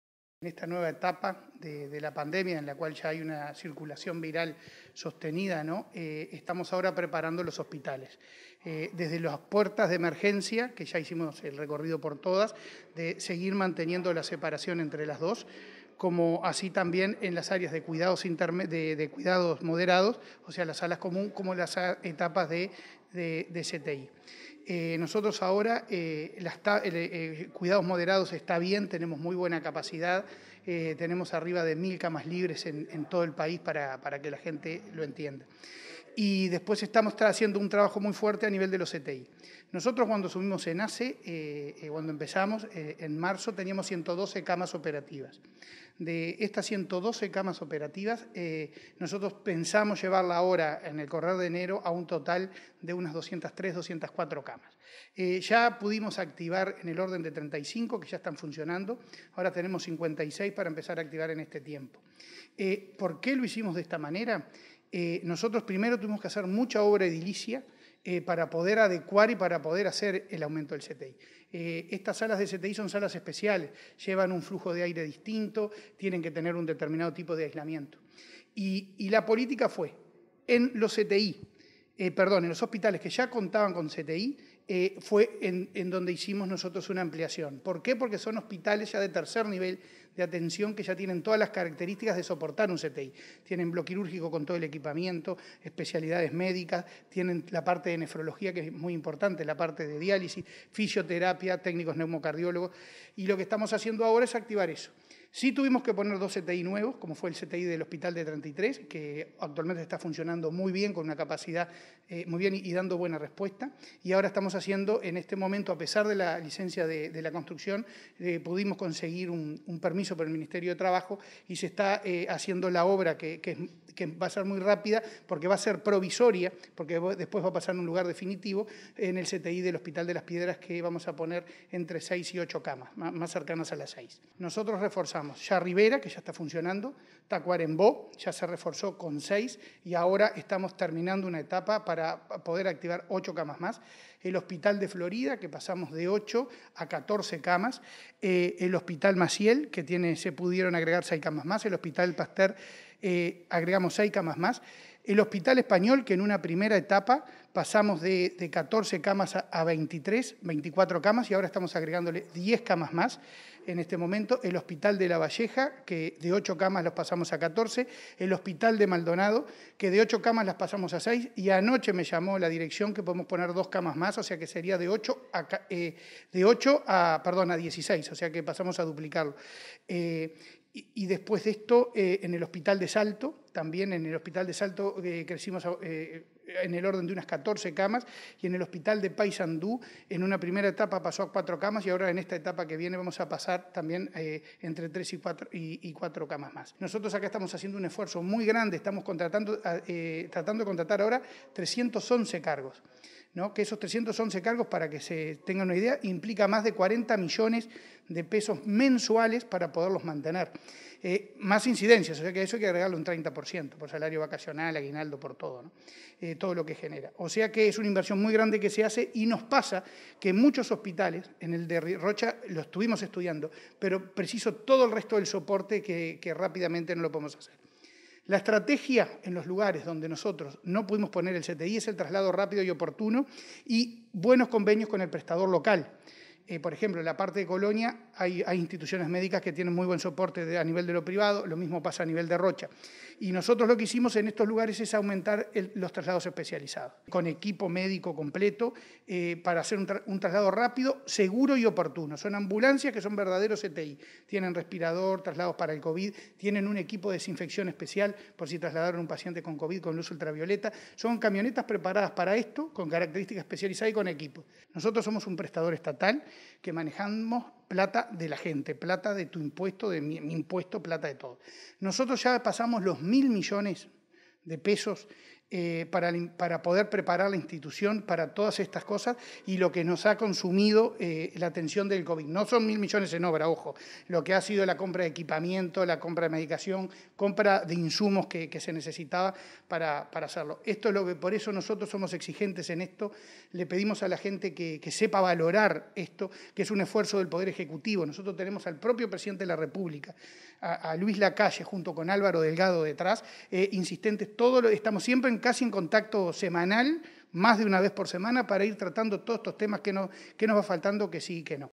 Terapia intensiva por Covid: En Rocha y Colonia se apela a traslados rápidos y oportunos
Para optimizar la atención durante la emergencia sanitaria, la Administración de los Servicios de Salud del Estado duplicará las camas de cuidados intensivos en varios puntos del país; llegará a 203 en enero próximo y adjudicará ambulancias especializadas donde no sea viable contar con CTI. Su titular, Leonardo Cipriani, dijo que la inversión en infraestructura, recursos humanos y materiales suma 1.000 millones de pesos.